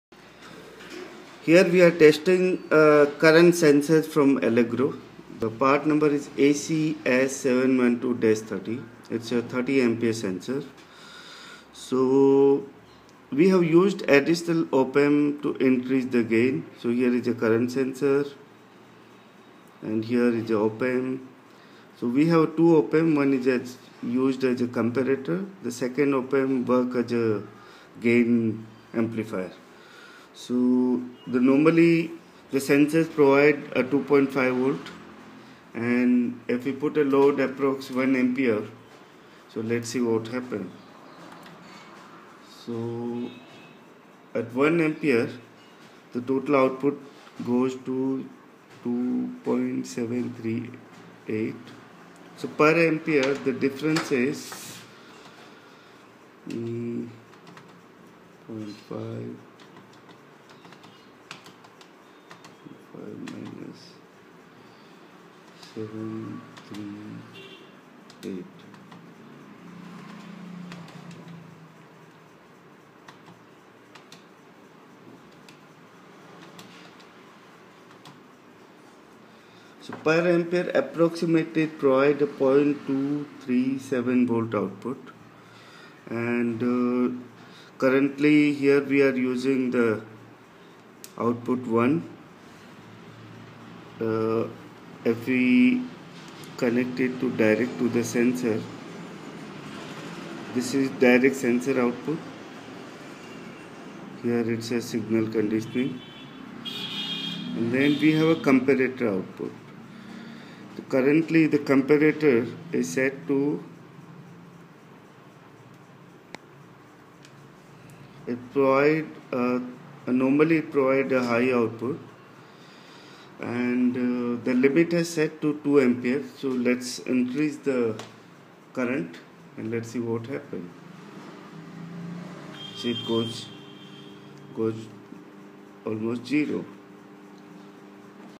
ACS712 30A SENSOR WITH VOLATGE AMPLIFIER AND FAULT OUTPUT
ACS712 30Amp current sensor with onboard op-amp amplifier and comparator for fault output or over current output.